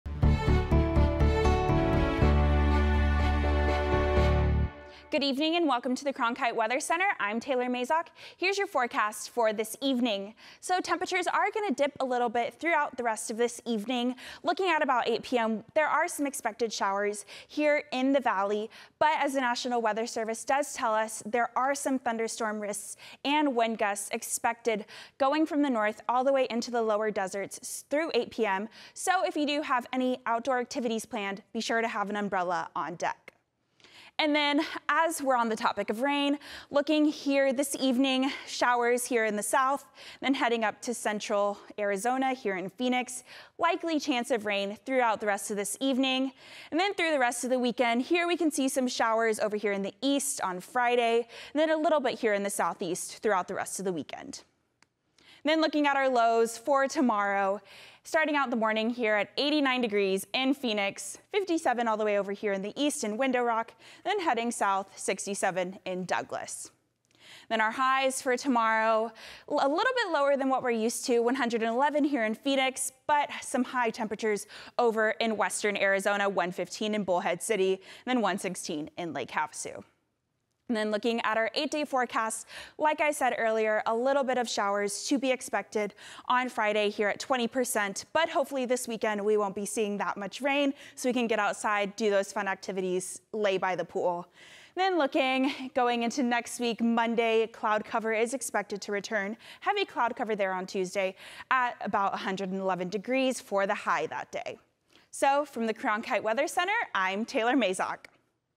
July 25, 2024 Weather
The warm temperatures will continue over the next few days but we also have a good chance at rain. (Video by Taylor Mazock/Cronkite News)